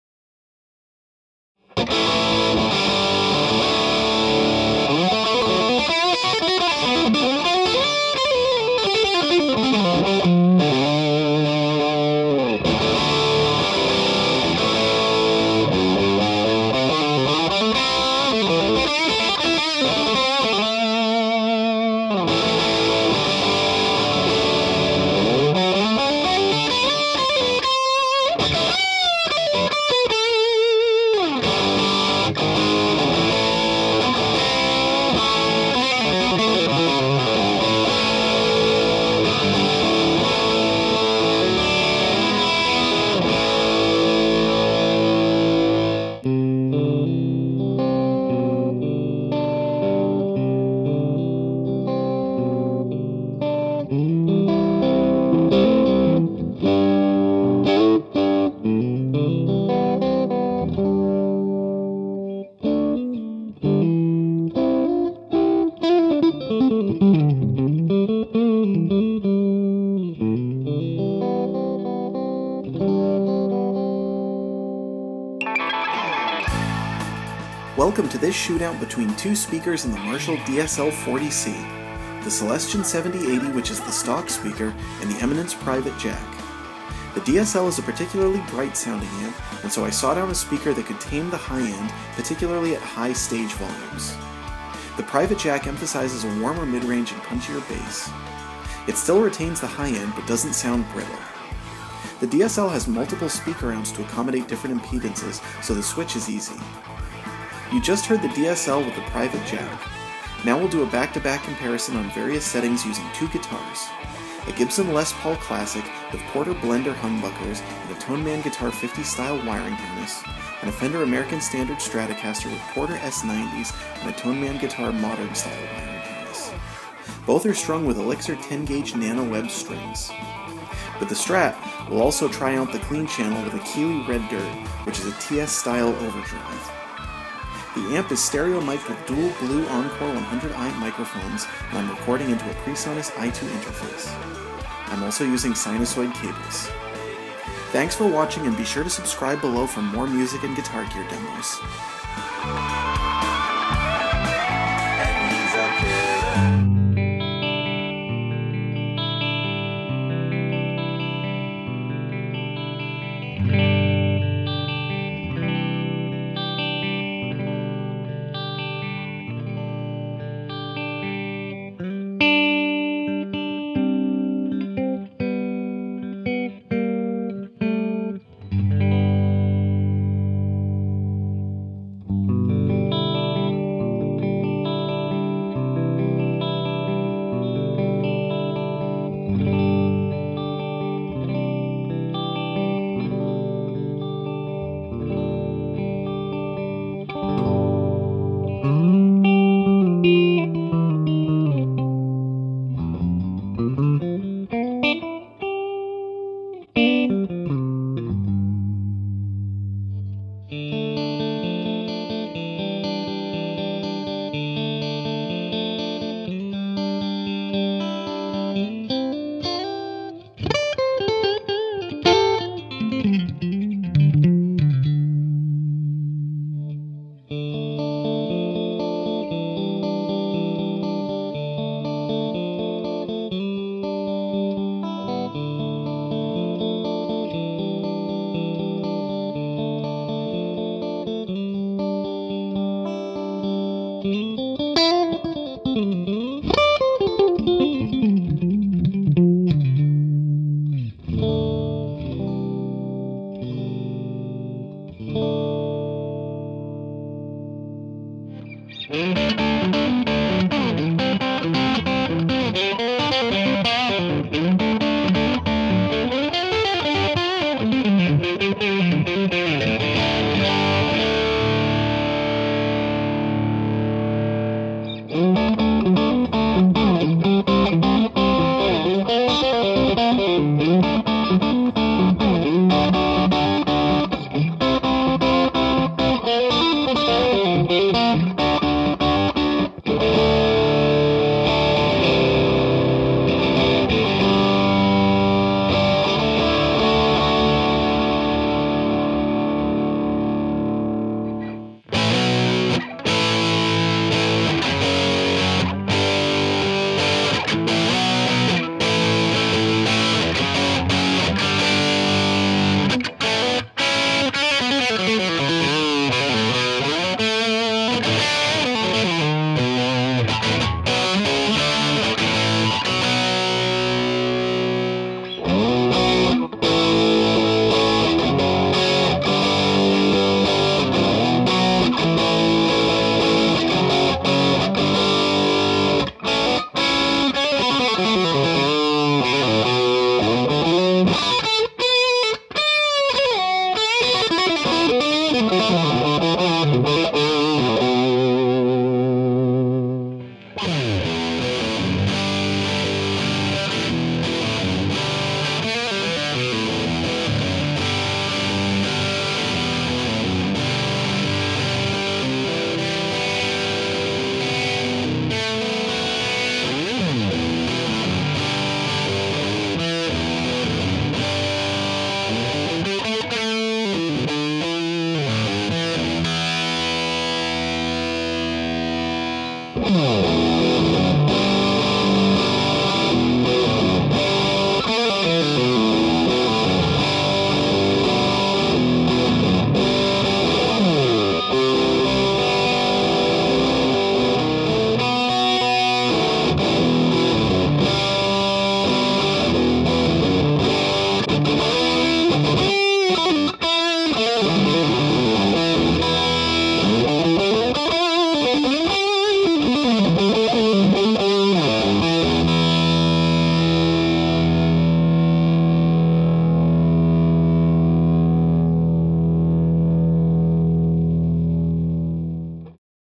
Marshall DSL40C Speaker Swap Eminence Private Jack vs Celestion Seventy 80
Eminence Private Jack vs Celestion Seventy 80 Marshall DSL40C Speaker Swap

Eminence Private Jack Eminence Red Coat Series Speaker Description from MusiciansFriend.com:
Thick, smooth, British flavor.

Perfect for blues, indie, and jazz. Lots of perfectly balanced mids and extended highs. Ideal in a 1, 2, or 4 x 12" open- or closed-back configuration. 50W RMS handling, 100W peak. 8 or 16 ohm impedance.

Eminence recommends the Red Coat Private Jack as a direct replacement speaker in the Behring Ultrastack BG412S & Ultrastack BG412H speaker cabinets. Eminence recommends the Private Jack-16 as a alternate replacement speaker in the Marshall 1960A & 1960B speaker cabinets.
FEATURES
Thick British tone for blues, indie and jazz music
Perfectly balanced mids and extended highs

Additional gear:
Gibson Les Paul Classic with Porter Blender Humbuckers & Tone Man Guitar 50's Style Wiring Harness
Fender American Standard Stratocaster with Porter S90s & Tone Man Guitar Modern Wiring Harness
Keeley Red Dirt Overdrive Pedal
Sinasoid Cables
Blue Encore 100i Microphones
Presonus iTwo Interface